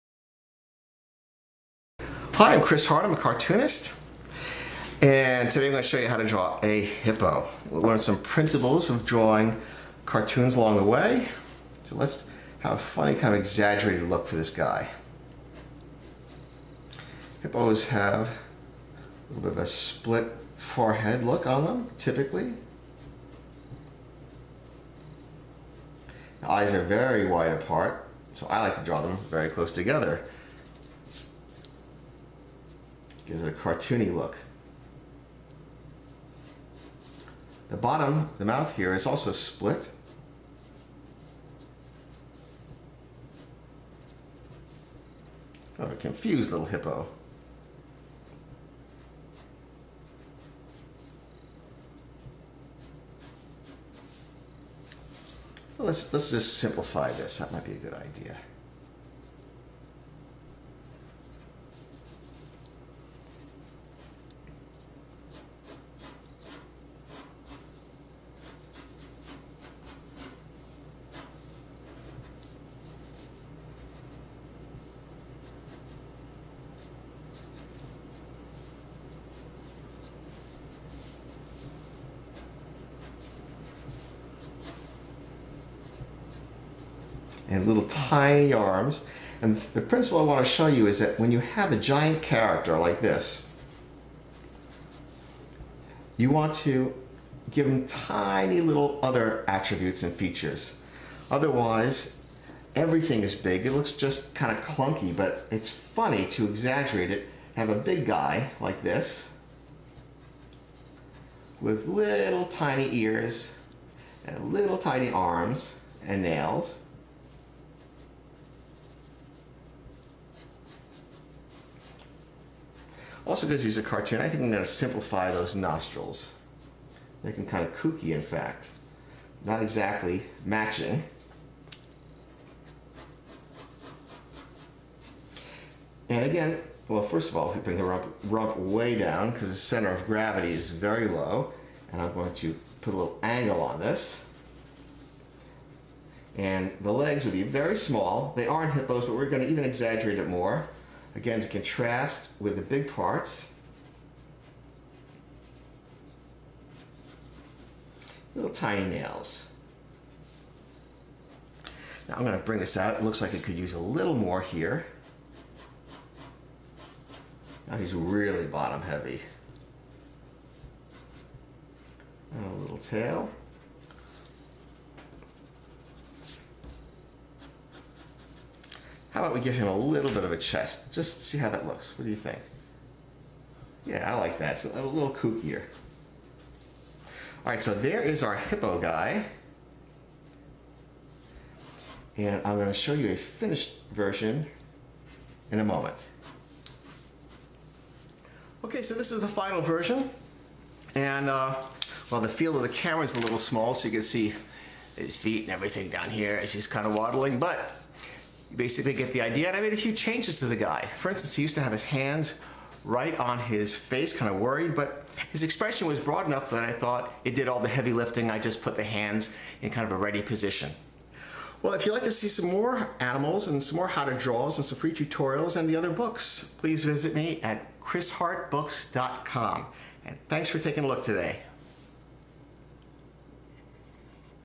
How To Draw a Hippo (Step by Step)
Free How to Draw a Hippo (Step by Step): Christopher Hart Shows How to Draw for Free. Watch Christopher Hart as he narrates a free lesson on how to draw a hippo character. Chris will teach you how to draw a cartoon hippo using pencil. Each of Christopher Hart's free video lessons provide great ideas using easy step by step tutorials so that you can learn to draw. Have fun as you sketch along with Chris. CAUTION: CHILDREN SHOULD USE CHILD-SAFE ART SUPPLIES.
Chris Hart shows you how to draw a super-appealing hippo cartoon character. Learn how to draw this very popular cartoon animal type.